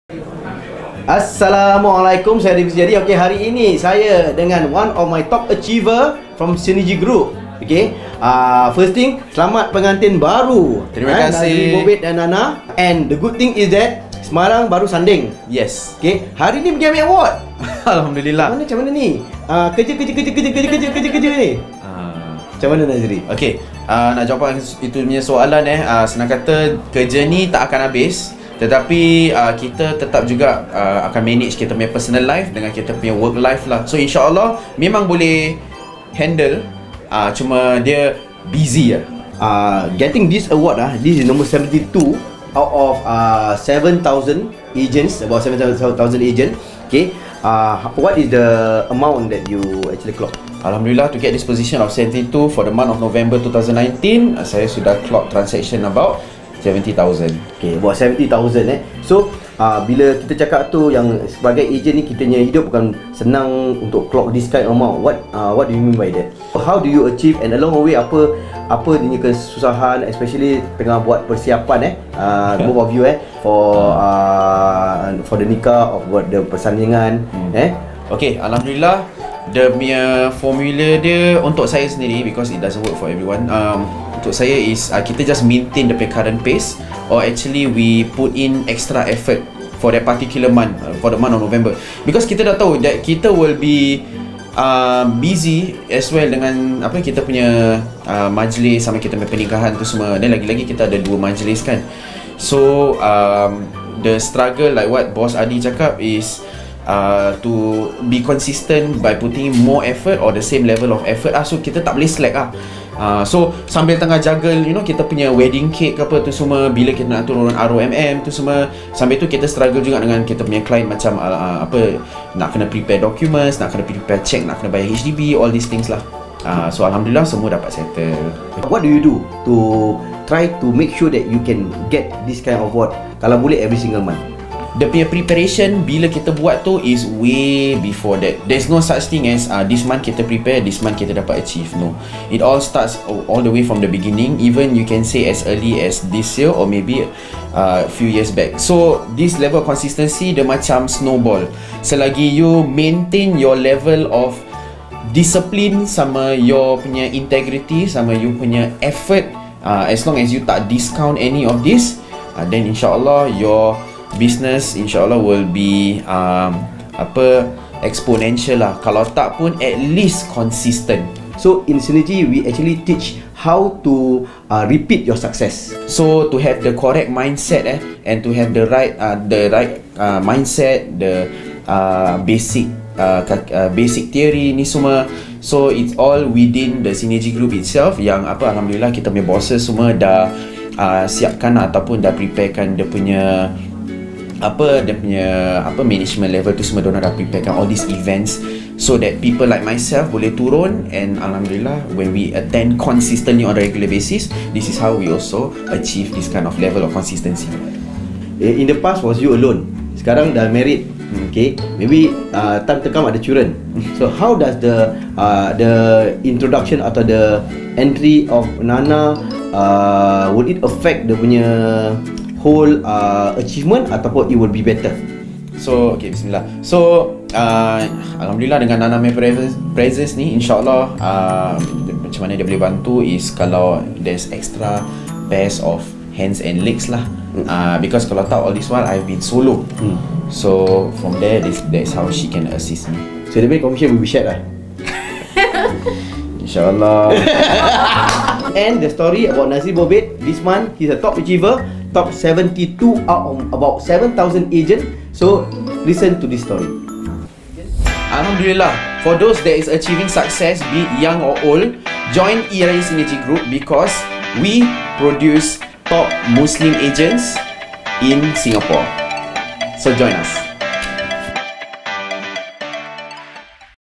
NAZRI BAOBED OUR TOP SYNERGY AGENTS!
Chit Chat Session with Nazri Baobed
one of our Top Malay Muslim Agents in Synergy Group in Era Level.

Lets listen to his successful story~!